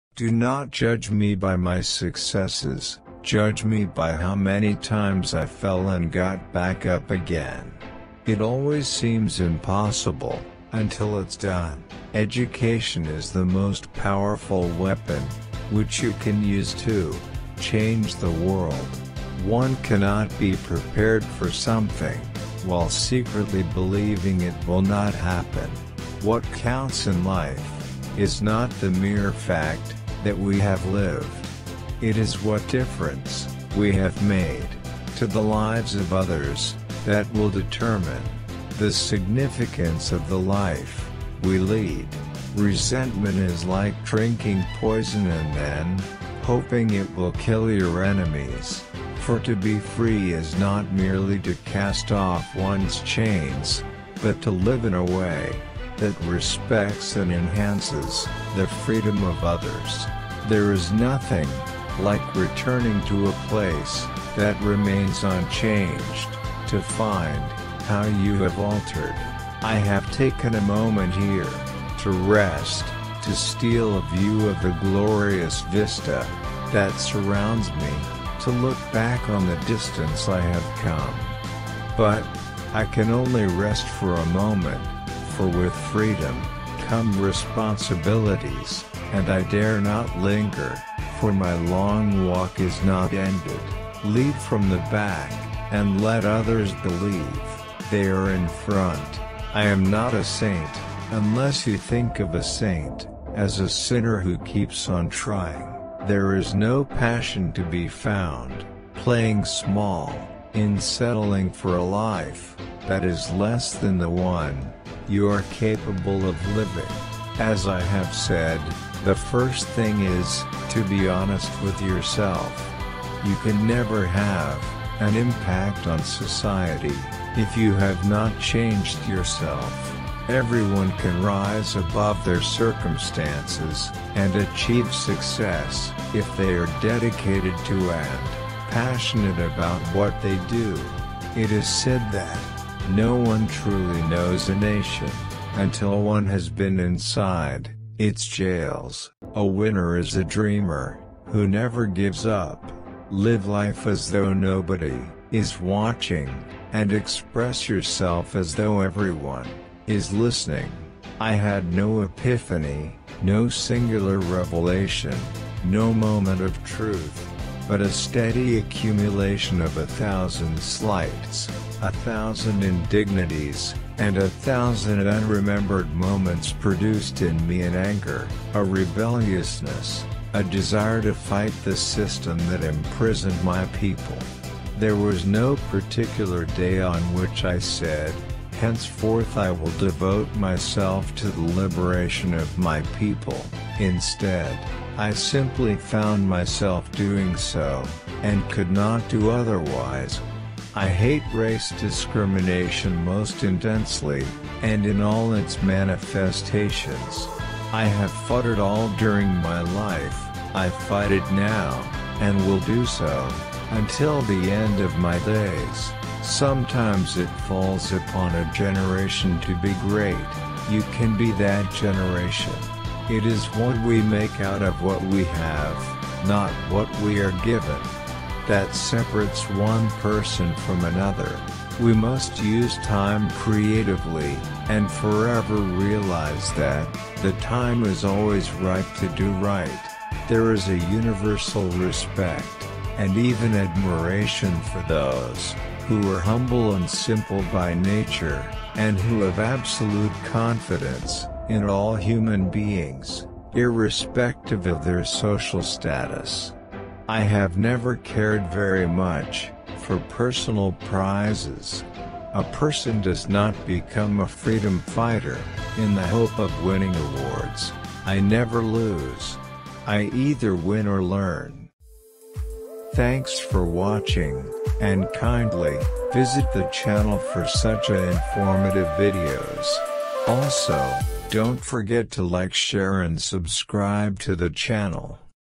25 Motivational Quotes: Nelson Mandela
Nelson Mandela was born in the village of Mvezo, on 18 July 1918. Nonqaphi Nosekeni and Nkosi Mphakanyiswa Gadla Mandela were his mother and father respectively. He died on 5 December 2013 in his home in Johannesburg.

Keep Visiting This Channel For Such an Informative Videos